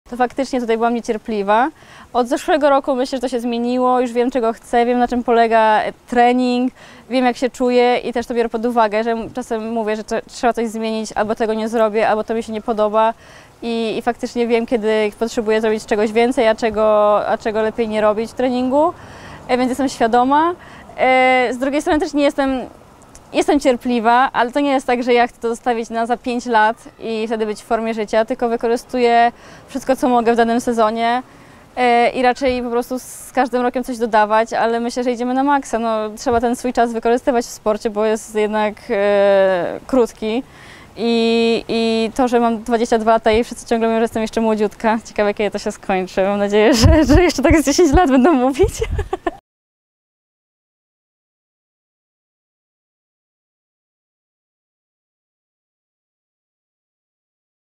"Wiem CZEGO CHCĘ. Idziemy na MAKSA" Pia Skrzyszowska: Ja w to WIERZĘ!
Pia Skrzyszowska zakończyła sezon 2023 bez medalu mistrzostw świata, ale jak sama przyznaje, teraz wie, czego chce i wie, na co ją stać. I to pomimo młodego wieku, który wiele osób jej podkreśla.